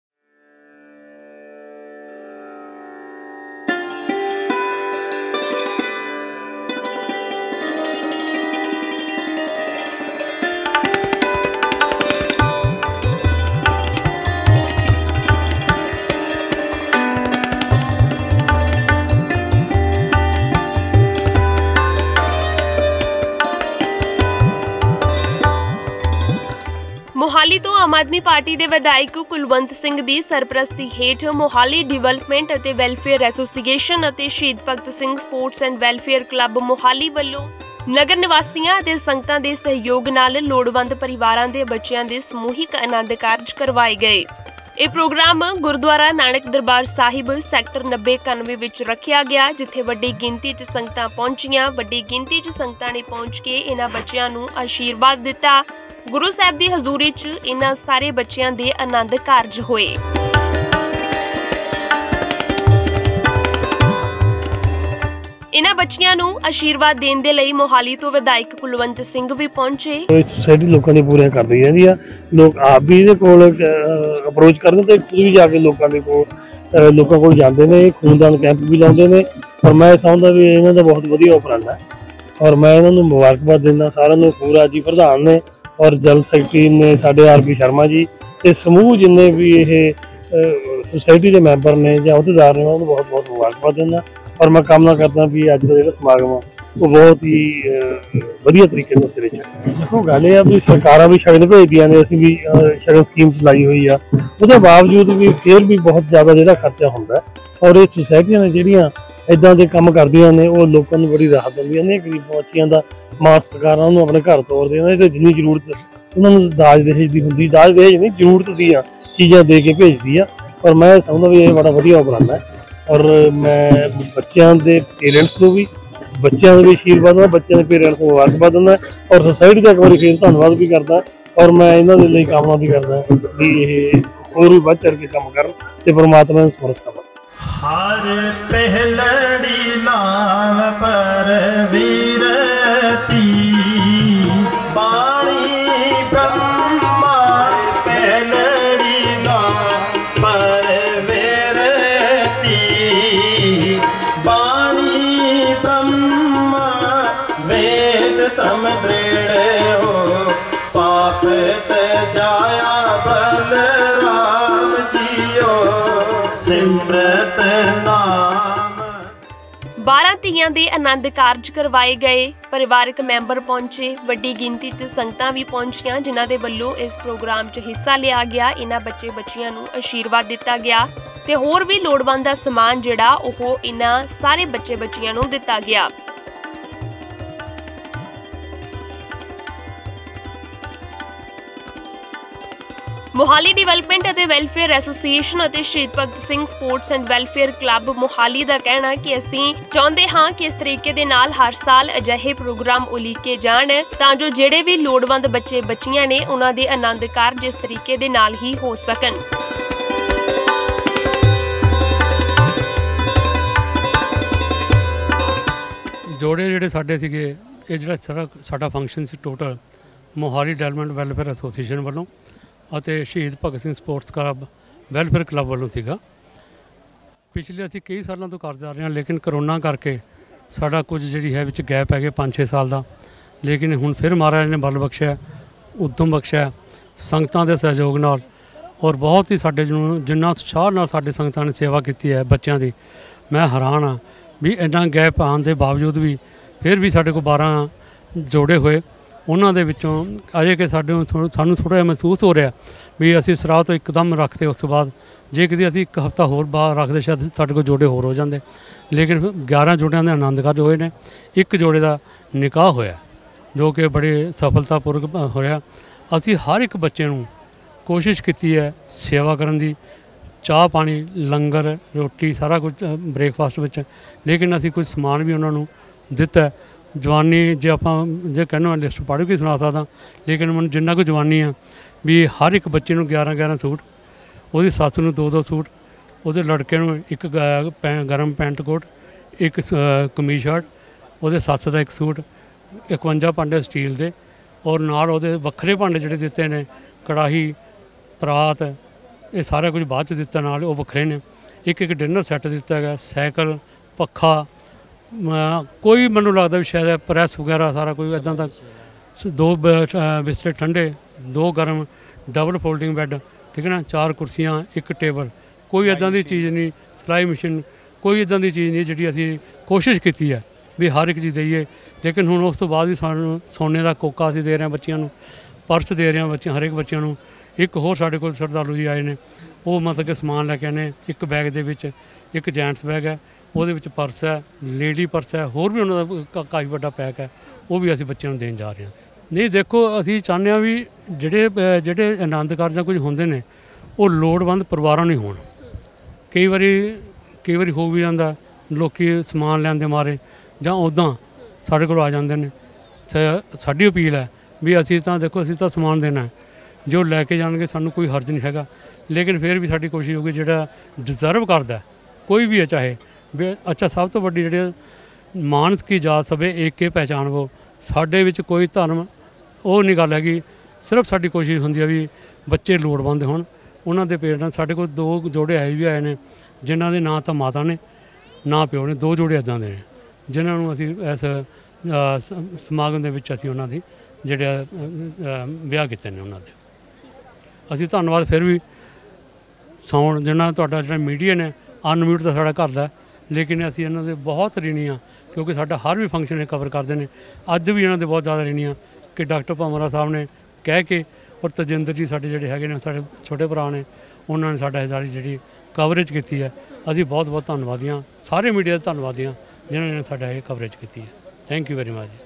ਮੋਹਾਲੀ ਡਿਵੈਲਪਮੈਂਟ ਤੇ ਵੈੱਲਫੇਅਰ ਐਸੋਸੀਏਸ਼ਨ, ਸ਼ਹੀਦ ਭਗਤ ਸਿੰਘ ਸਪੋਰਟਸ ਐਂਡ ਵੈੱਲਫੇਅਰ ਕਲੱਬ ਨੇ ਕਰਵਾਏ ਅਨੰਦ ਕਾਰਜ
ਮੋਹਾਲੀ ਡਿਵੈਲਪਮੈਂਟ ਅਤੇ ਵੈੱਲਫੇਅਰ ਐਸੋਸੀਏਸ਼ਨ ਤੇ
ਸ਼ਹੀਦ ਭਗਤ ਸਿੰਘ ਸਪੋਰਟਸ ਐਂਡ ਵੈੱਲਫੇਅਰ ਕਲੱਬ ਵੱਲੋਂ
ਲੋੜਵੰਦ ਬੱਚਿਆਂ ਦੇ ਕਰਵਾਏ ਗਏ ਅਨੰਦ ਕਾਰਜ
MLA ਕੁਲਵੰਤ ਸਿੰਘ ਨੇ ਪਹੁੰਚ ਕੇ ਦਿੱਤਾ ਅਸ਼ੀਰਵਾਦ